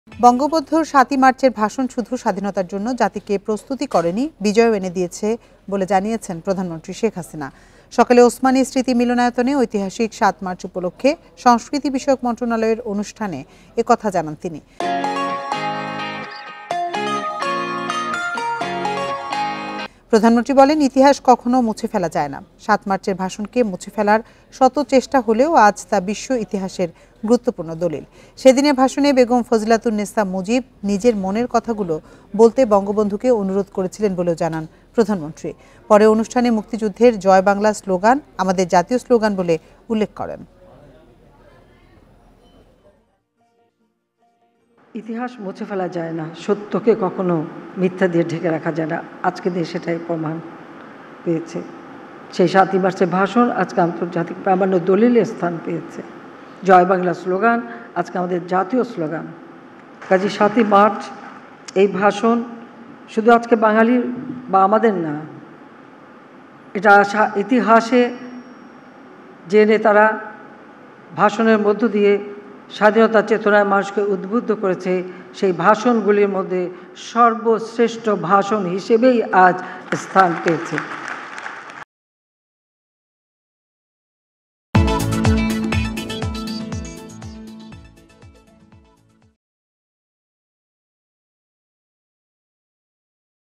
‘মুক্তিযুদ্ধের জয় বাংলা স্লোগান আমাদের জাতীয় স্লোগান’ | 7 March Speech | Independent TV
বঙ্গবন্ধুর ৭ই মার্চের ভাষণ শুধু স্বাধীনতার জন্য জাতিকে প্রস্তুতই করেনি, বিজয়ও এনে দিয়েছে বলে জানিয়েছেন প্রধানমন্ত্রী শেখ হাসিনা। সকালে ওসমানী স্মৃতি মিলনায়তনে ঐতিহাসিক ৭ মার্চ উপলক্ষে সংস্কৃতি বিষয়ক মন্ত্রণালয়ের অনুষ্ঠানে এ কথা জানান তিনি। প্রধানমন্ত্রী বলেন, ইতিহাস কখনো মুছে ফেলা যায় না। ৭ মার্চের ভাষণকে মুছে ফেলার শত চেষ্টা হলেও আজ তা বিশ্ব ইতিহাসের গুরুত্বপূর্ণ দলিল।

Prime Minister Sheikh Hasina said Bangabandhu's March 7 speech not only prepared the nation for independence but also brought victory. He said this at the ceremony of the Ministry of Culture on the occasion of historical March 7 at the Osmani Memorial Auditorium in the morning. The Prime Minister said that history can never be erased. Although hundreds of attempts have been made to erase the March 7 speech, today it is an important document of world history.